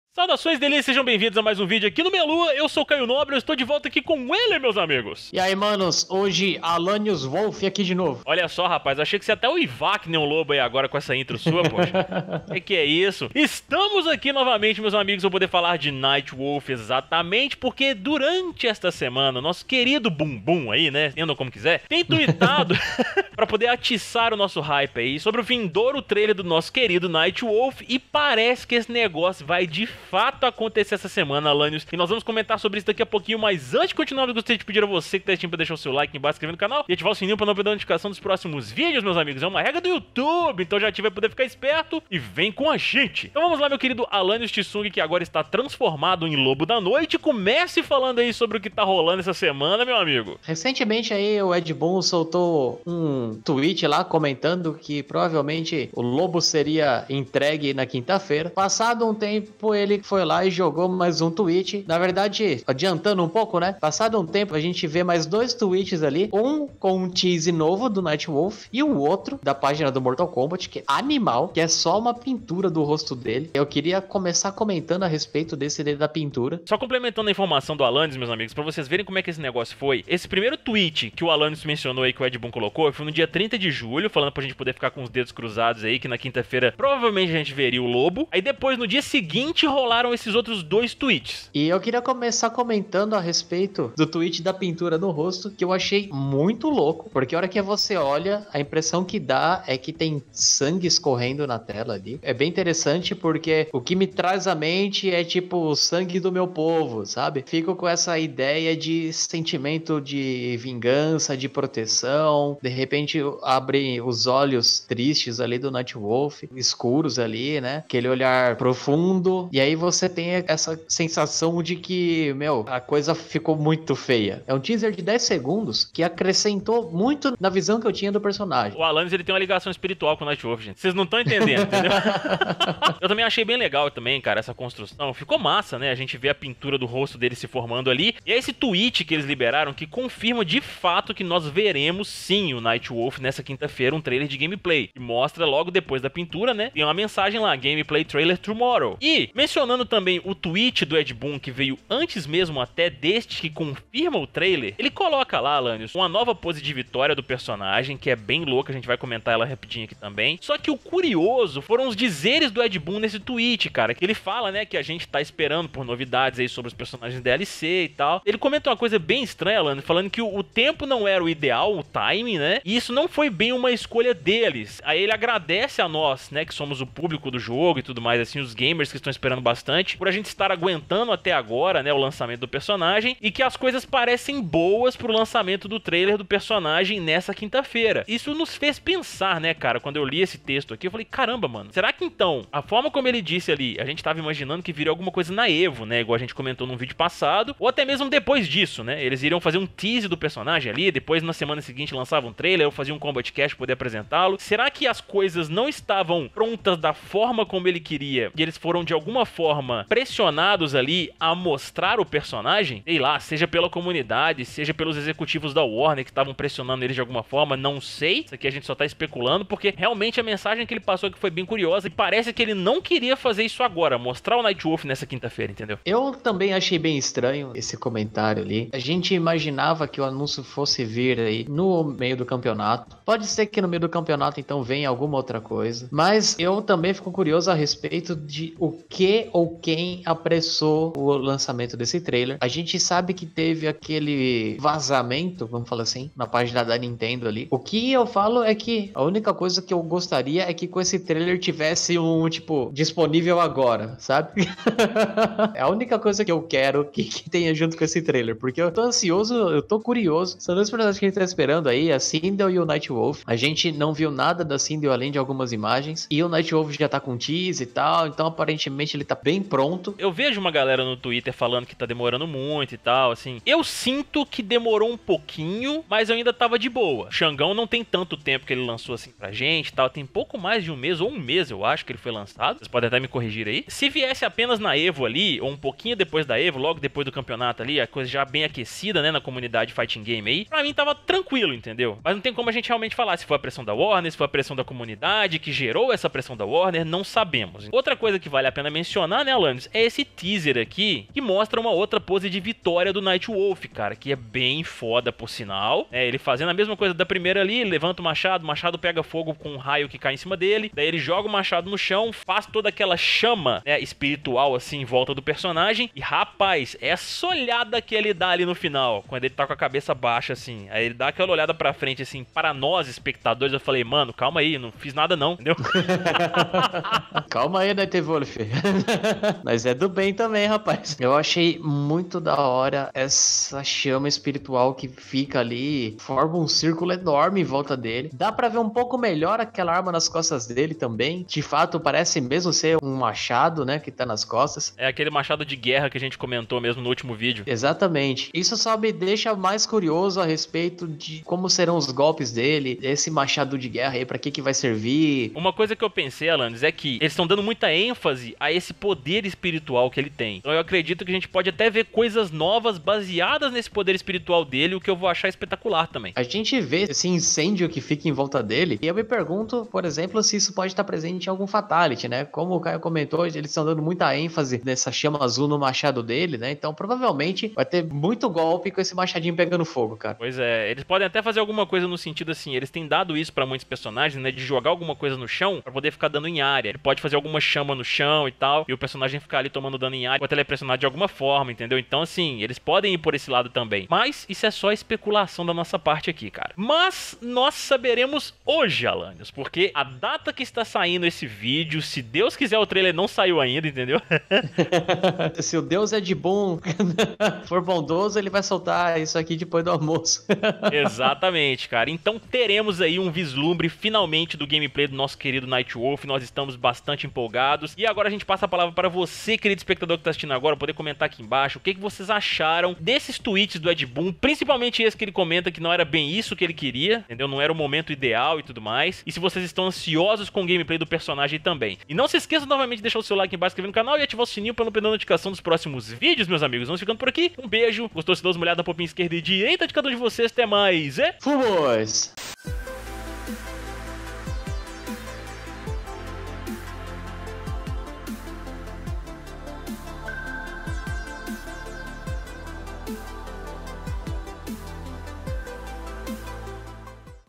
MORTAL KOMBAT 11: NIGHTWOLF CHEGA NESTA QUINTA COM MENSAGEM CURIOSA DE ED BOON
Hoje comentamos sobre o teaser do grande Nightwolf que terá seu trailer liberado nesta quinta-feira e também sobre uma mensagem curiosa deixada pelo Ed Boon em seu twitter.